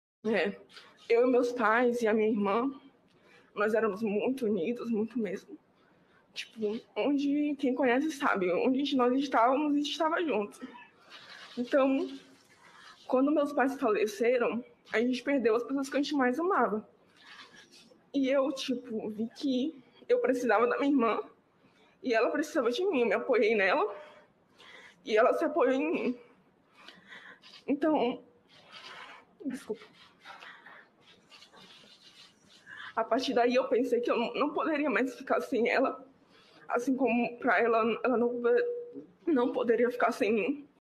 Interprete de libras se emociona em relato na CPI da Covid
CPI DA COVID: INTERPRETE DE LIBRAS SE EMOCIONA NO RELATO DE JOVEM QUE PERDEU OS PAIS

Nem mesmo o interprete de libras conseguiu se conter com o depoimento de Giovanna Gomes, jovem de 19 anos que perdeu o pai e mãe pra Covid-19, e agora assumirá a responsabilidade de ter a guarda da irmã de 11 anos. Talvez essa tristeza teria sido evitada se o próprio presidente não tivesse ignorado tantas ofertas de vacinas e incentivado as medidas sanitárias recomendadas pela comunidade científica internacional.